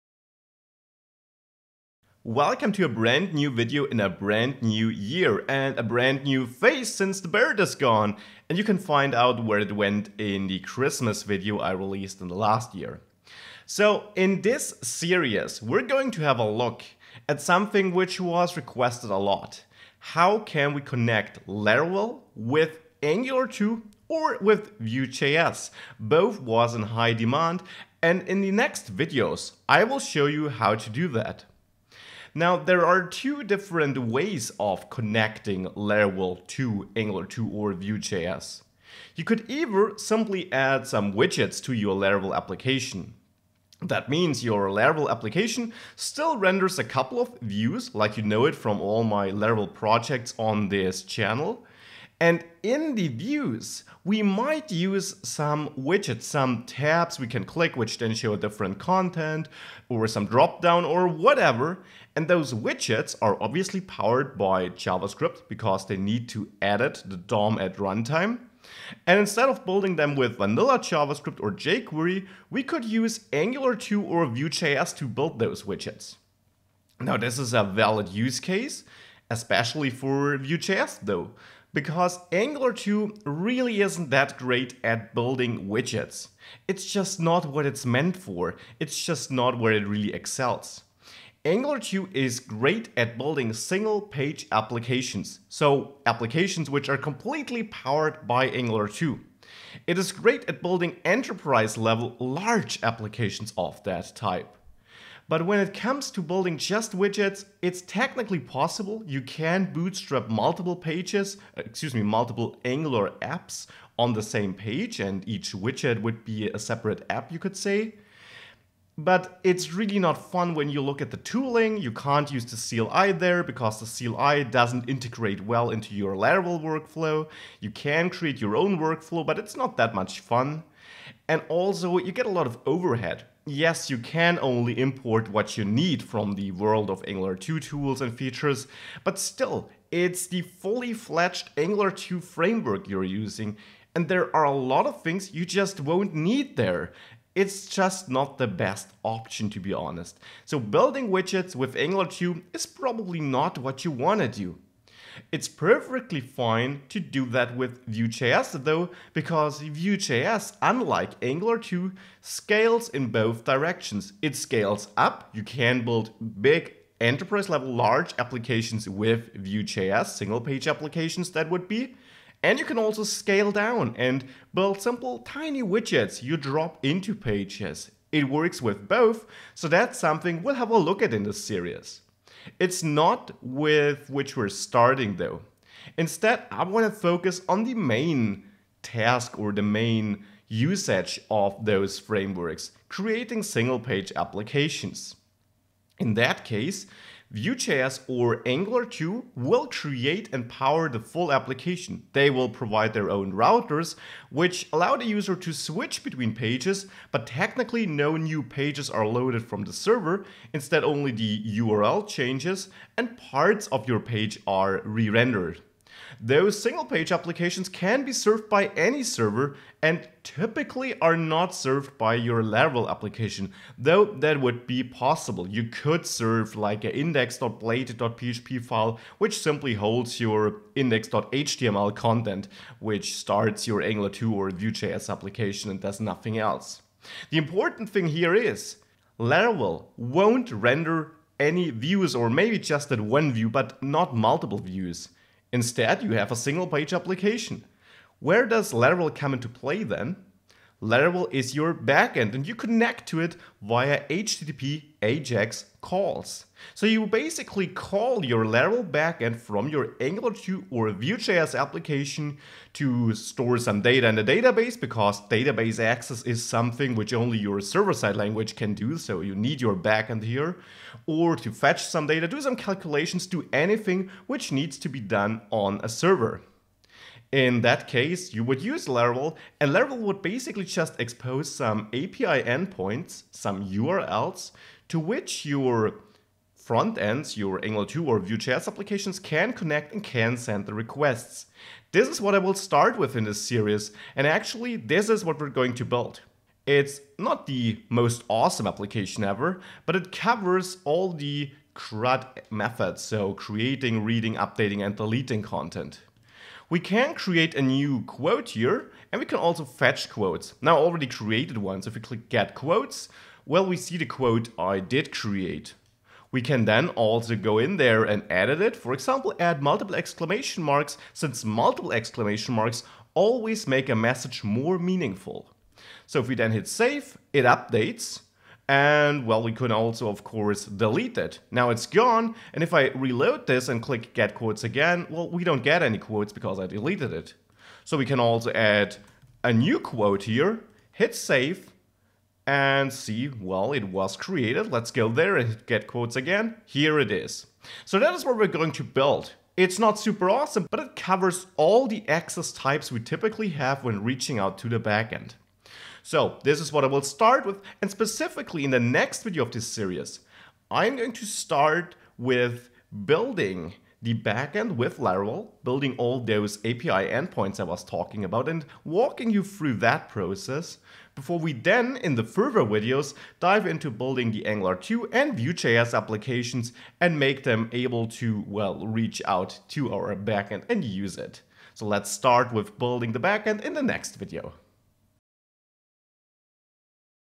THEORY | Laravel + Angular 2 / Vue.js 2
Learn how to use Laravel and Angular or Vue.js together. This videos dives into the theory behind this and how you could connect these frameworks.

The full source code can be found in the following Github repo (choose the right branch!)